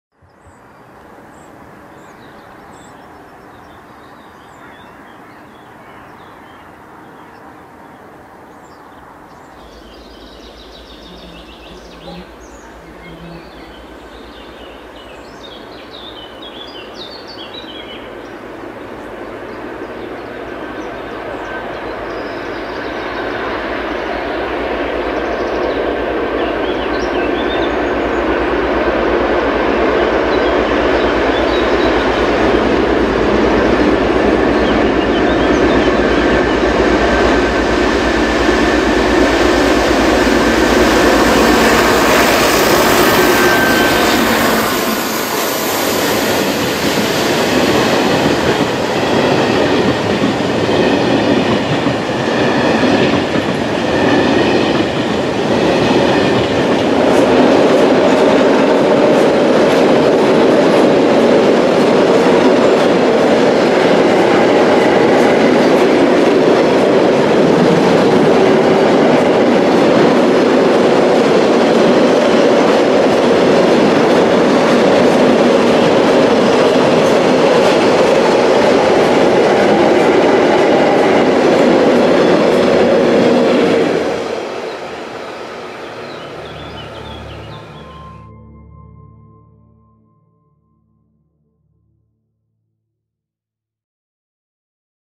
Ganz viele Neu- Pkw! Ludi 233 521 bringt sie kraftvoll bergwärts aus Tschechien nach Marktredwitz
Die Oberpfalz ist eine der wenigen Regionen in Deutschland, in denen man noch mehrmals täglich verschiedene "Ludmillas", also Dieselloks der Baureihen 232 und 233 im täglichen Dienst erleben kann.
Im Streckenkreuz aus Nord-Süd und Ost-West Richtung liegt Marktredwitz.
Hier treffen sich die Güterzüge aus den verschiedenen Richtungen.
Wichtige Achse im Güterverkehr ist die Bahnstrecke aus Cheb, Tschechien. Mehrmals täglich gibt es Güterzüge von und nach dort, neben den Zügen von DB Cargo fahren auch private Anbieter mit. Der bekannteste dürfte der "Schrott" sein von/ zum Stahlwerk Thüringen in Könitz bei Saalfeld, der ebenfalls hier entlang fährt. An meinem Besuchstag gab es tagsüber keine richtig passende Leistung, aber über den Sonnenstand musste ich mir dann sowieso keine Gedanken mehr machen...
Ein eingelegter Zug brachte mir die 233 521 vor die Kamera, die ihren Neuwagenzug kräftig den Berg hinauf zieht. Sie passiert gerade den 96m langen Elisenfelser Viadukt über die Röslau und wird in ca. 9 Kilometern Marktredwitz erreicht haben.
Aktuell möchte man langsam auf die ECR Class 77 umstellen, diese verfügen aber noch nicht über eine Zulassung in Tschechien, daher sind die 232/ 233 hier noch eine "sichere Bank". Auch sind noch nicht ausreichend der 247.0 verfügbar, um alle sonstigen Leistungen abdecken zu können, die bisher eingesetzten Ludis werden also weiterhin zu sehen sein.
Viel Spaß mit der röhrenden Diesellok, liebe Grüße euer Torsten!
16062015